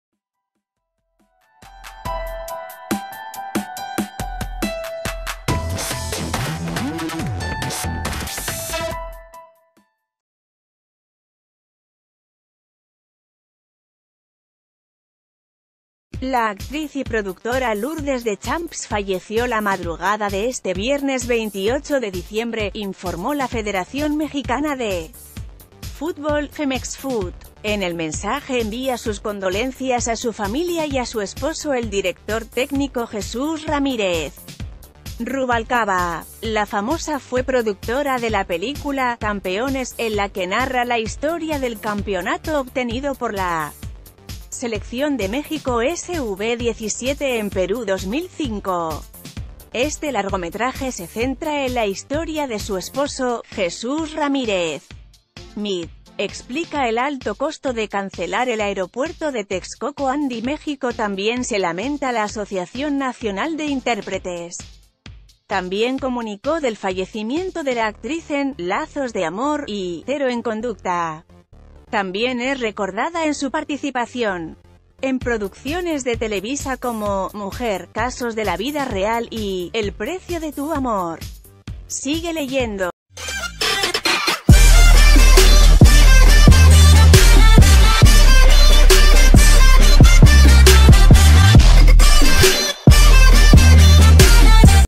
Lamentan fallecimiento de actriz Lourdes Deschamps y esposa de Director Técnico Jesús Ramírez Ruv...
Lamentan fallecimiento de actriz Lourdes Deschamps y esposa de Director Técnico Jesús Ramírez Ruvalcaba

La actriz y productora Lourdes Deschamps falleció la madrugada de este viernes 28 de diciembre, informó la Federación Mexicana de Fútbol (Femexfut). En el mensaje envía sus condolencias a su familia y a su esposo el Director técnico Jesús Ramírez Ruvalcaba. La famosa fue productora de la película "C...